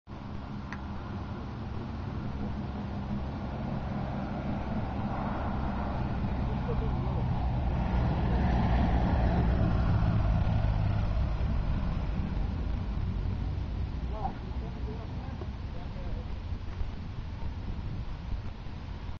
Cessna 172, N769GW Landing at AeroCountry T31 on 12-17-2011 at 1551
Cessna 172 landing at AeroCountry Airport